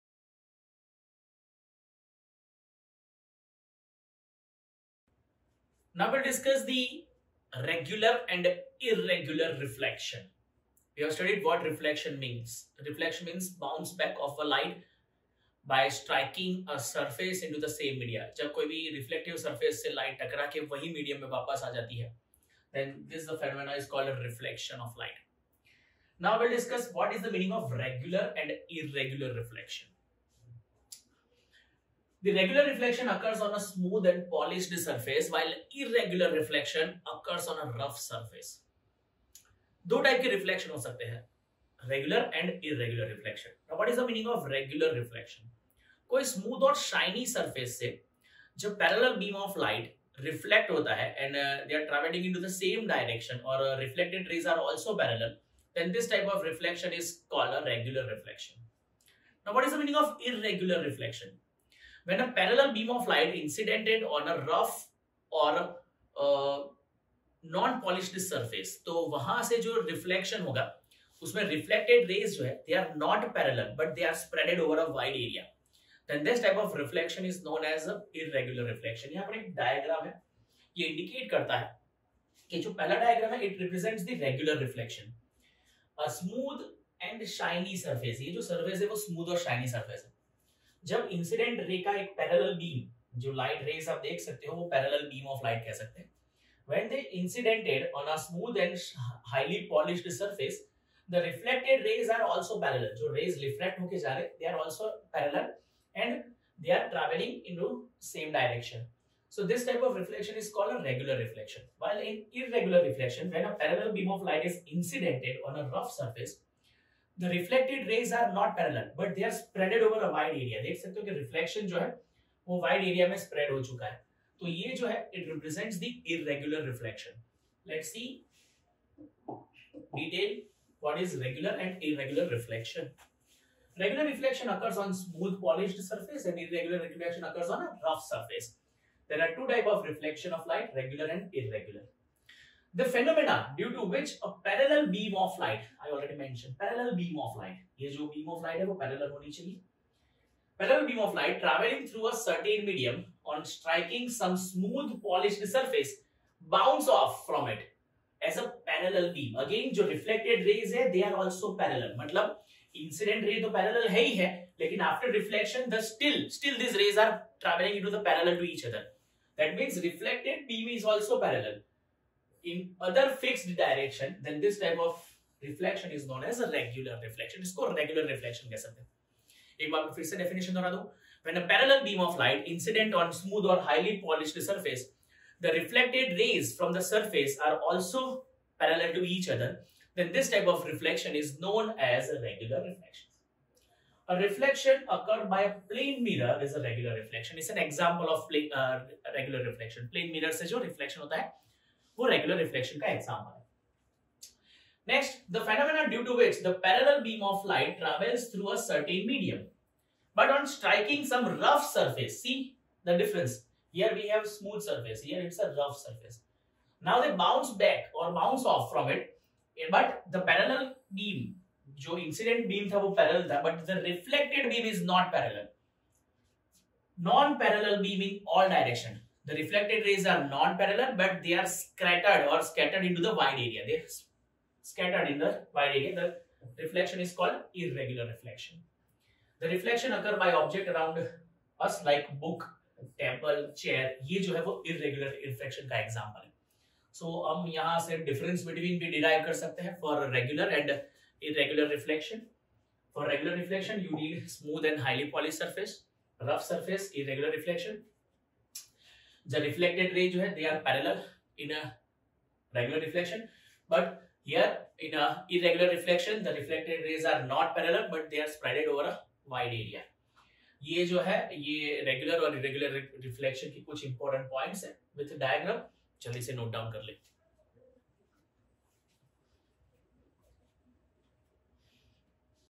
Regular and Irregular Reflection Chapter 10 (Light - Reflection & Refraction) | Class - 10th | NCERT
What is Regular and Irregular Reflection | Science | Chapter 10th - Light Reflection & Refraction.